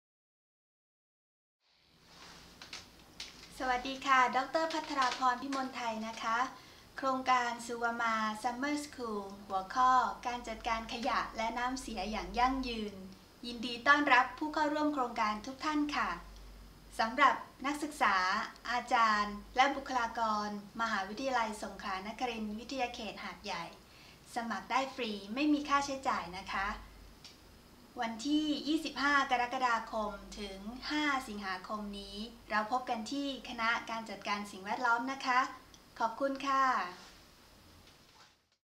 Welcoming invitation to SUWAMA.EU Summer School 2016
A short invitation to participants for SUWAMA.EU Summer School 2016 in Thai language. From 25th-29th July and 1st-5th August 2016. We wish you enjoyable and fruitful Summer School.